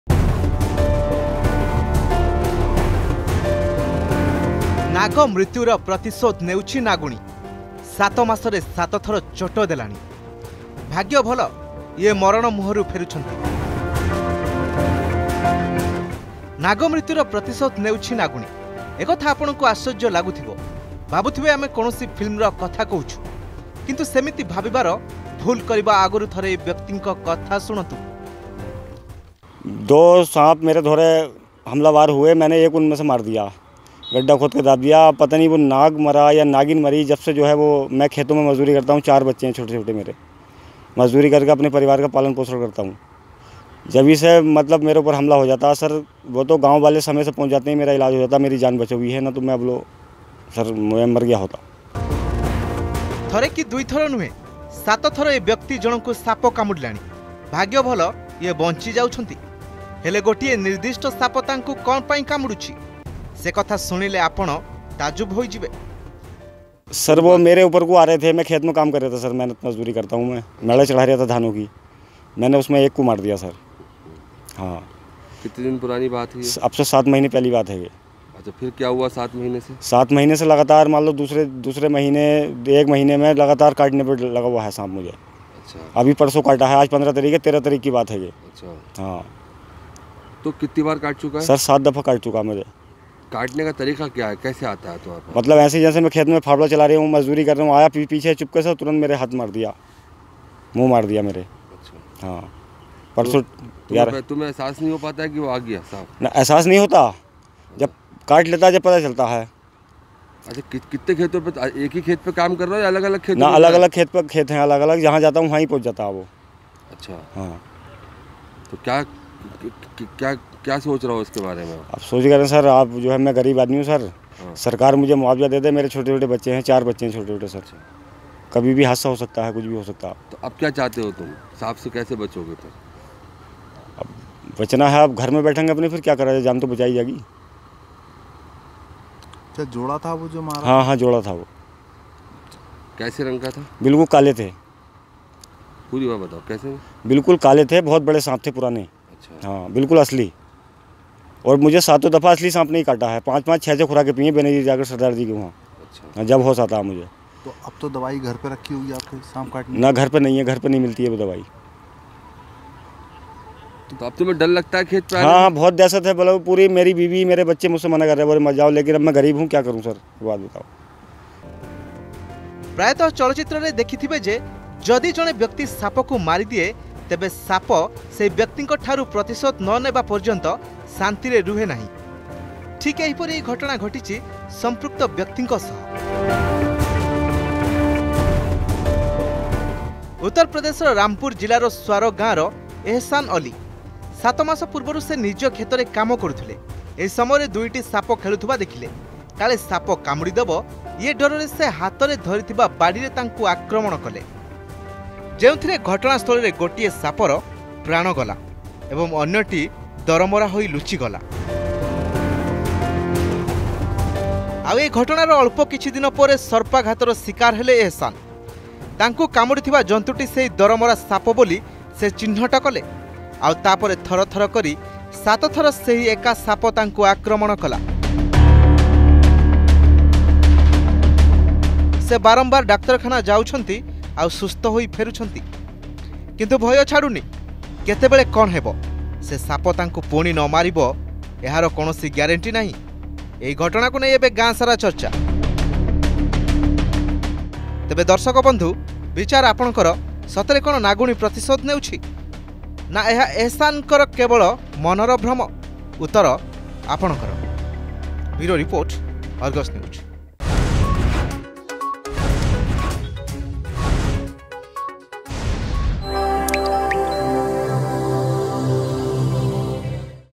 Snake takes revenge, bites a man 7 times in Uttar Pradesh
A shocking case has come to light in Uttar Pradesh, where a female snake bites a man 7 times to avenge the murder of the male snake, but the man survived each time.